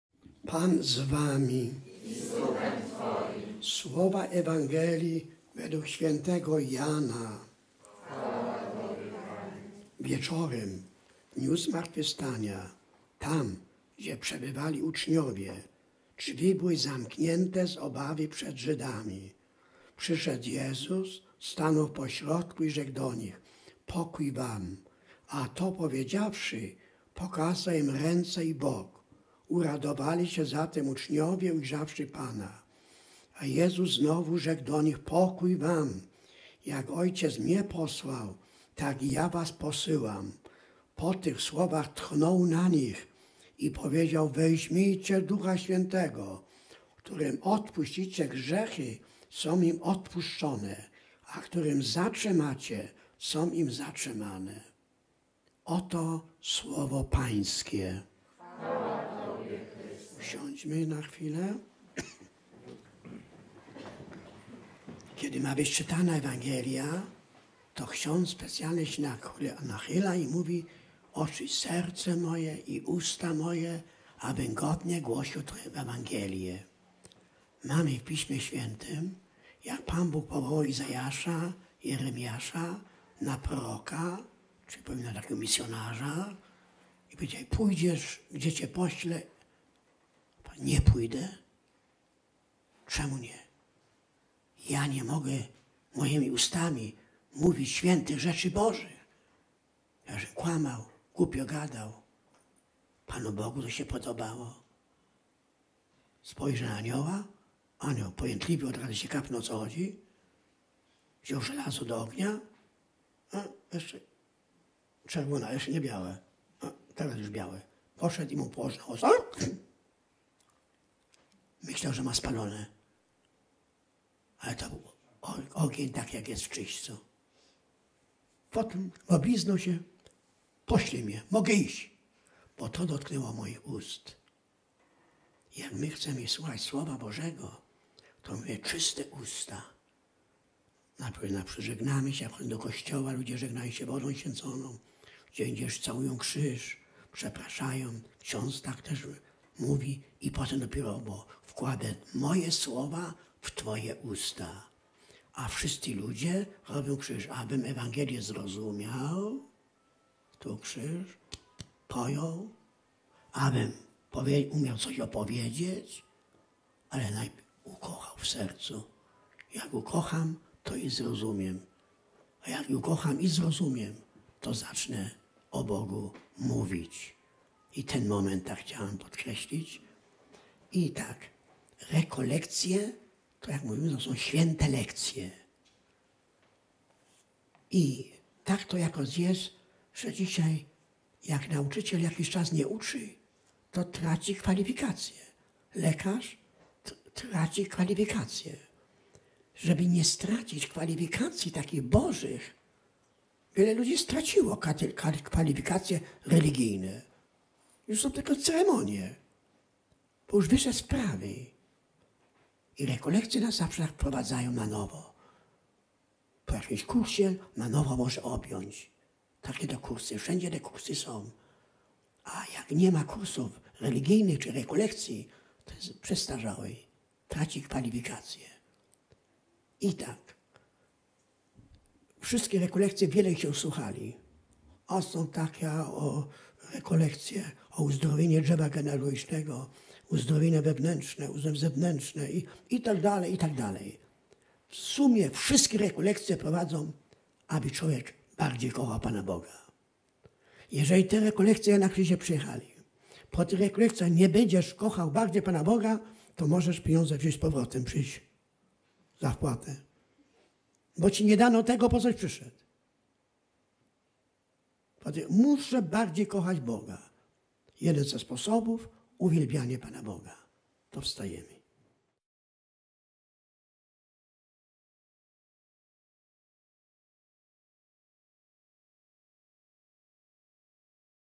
Rekolekcje „Moc uwielbienia” w Toruniu 24.10.2014 - Początek
Rekolekcje u franciszkanów w Toruniu 24 do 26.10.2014 „Moc uwielbienia”
Prowadzą: O. Błażej Sekula i Małgorzata Heigelmann